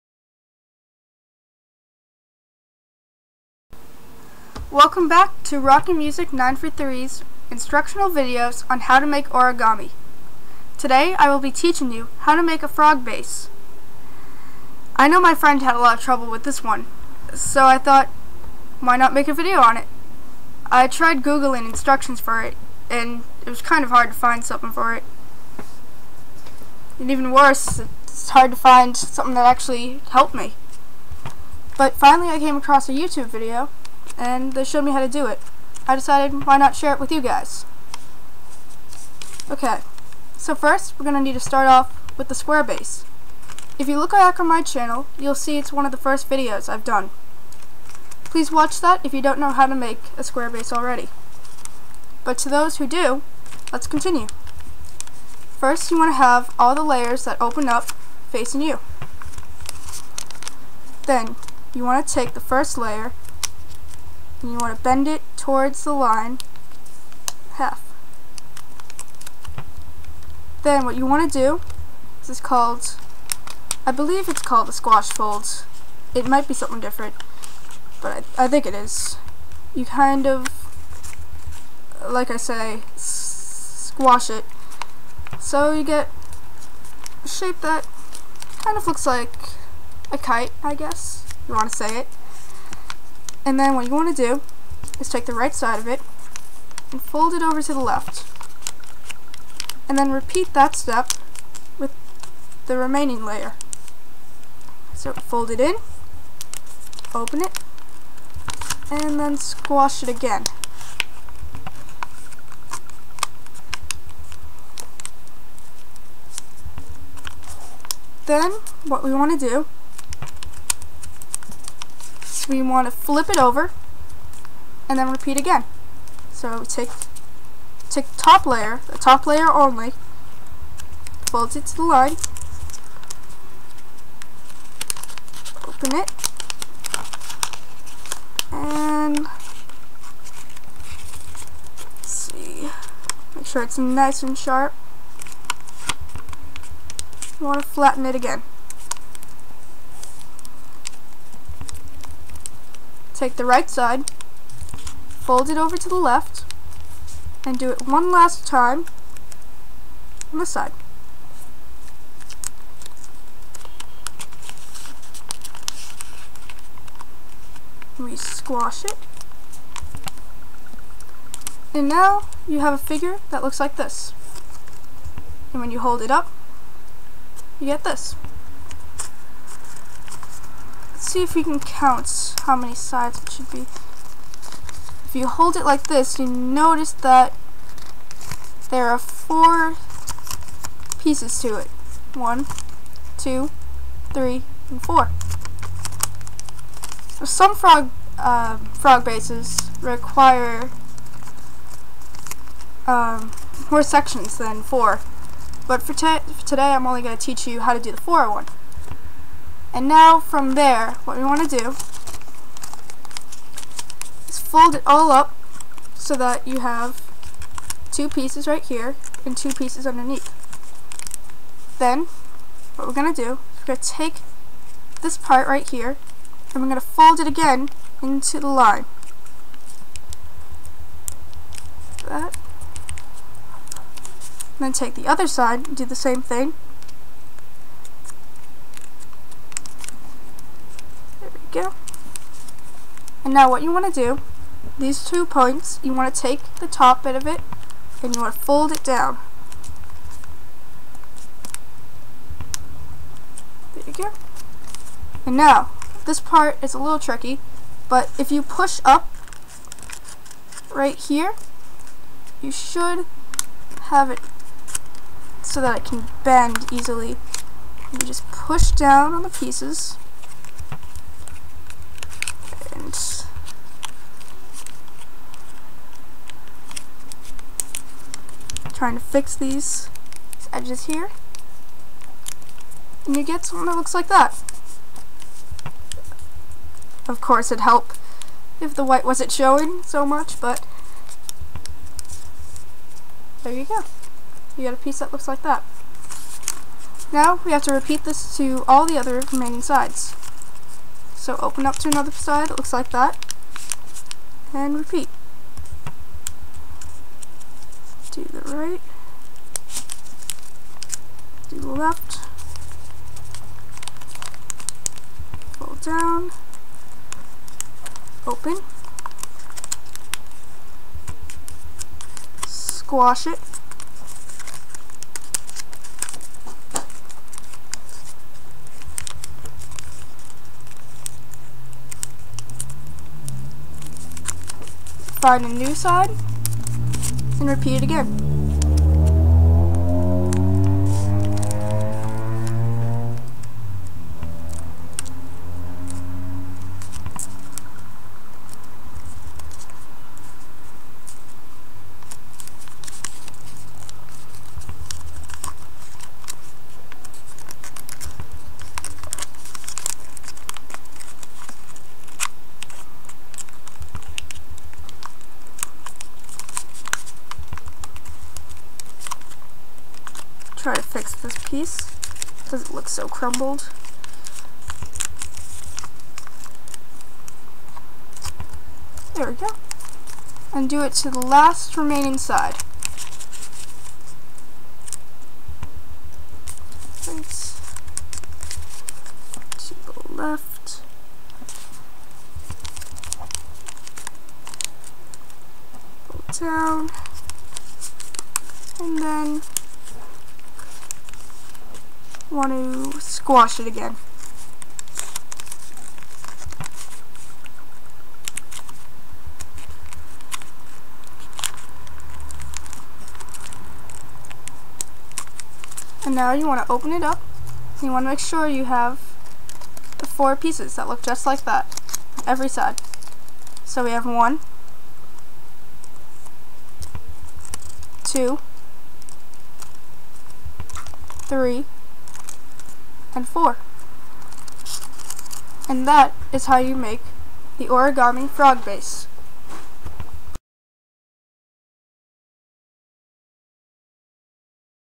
Origami 101: Frog Base
In this video I teach you how to make a frog base. This type of base is used to make slightly more complicated pieces.